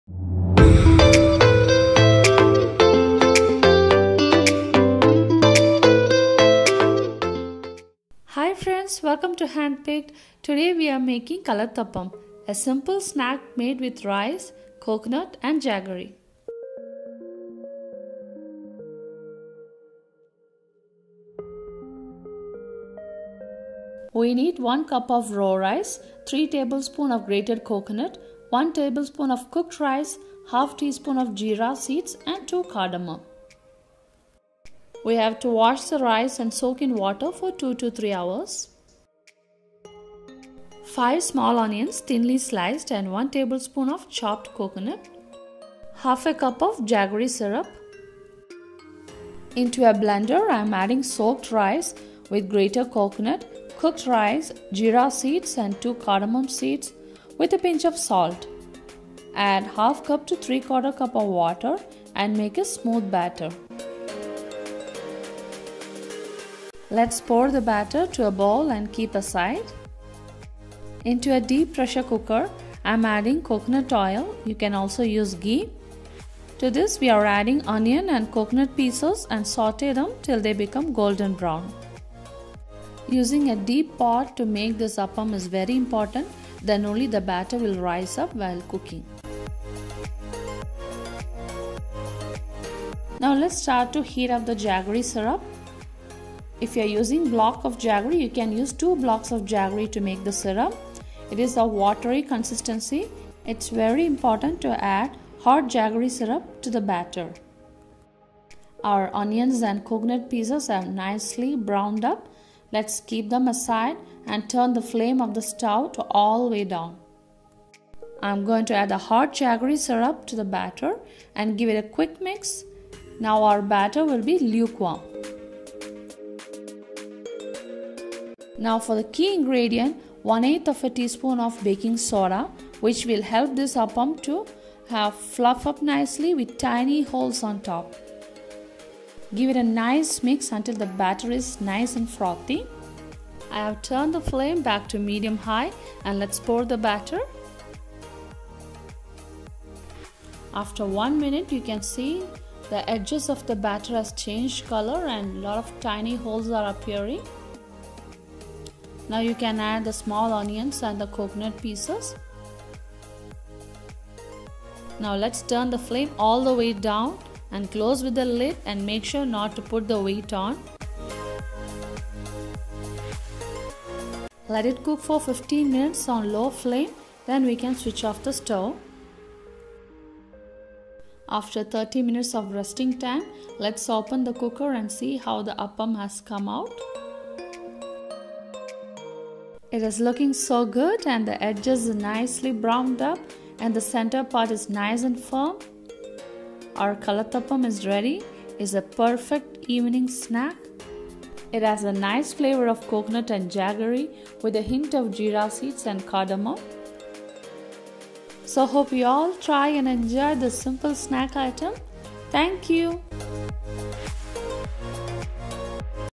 KALATHAPPAM evening snack... just 3 ingredients
A perfect snack made with raw rice, coconut and jaggerry flavoured with
jeera seeds n cardamom

Ingredients

Raw rice/Jeera rice     1 cup
Grated coconut          3 tbsp
Cooked rice               1 tbsp
Jeera seeds              1/2 tsp

Jaggery syrup           1/2 cup

*If using jaggery squares

 2 jaggery square with 1/4 c of water

Small onions      5,thinly sliced
Coconut pieces    1 tbsp

Coconut oil/Ghee 3 tbsp

Preparation:

1. Wash n soak for 3 hours
2. Grind soaked rice, grated coconut, cooked rice, jeera seeds, cardamom
   with a pinch of salt n water (1/2 c to 3/4 c)
3. Keep aside
4. Boil jaggery syrup
5. Heat deep pressure cooker, saute onions n coconut pieces.
6. Add hot jaggery syrup to batter, give a quick mix
7. Add baking soda n mix nicely to form froth on top
8. Keep flame to medium high and pour batter
9. After 1 min, add fried onions n coconut on top
10. Turn flame to low, close with lid ( no weight )
11. Cook for 15 mins on low flame
12. Rest for 30 mins, open the cooker
13. Kalathappam is ready

Serving size: 6 to 8 persons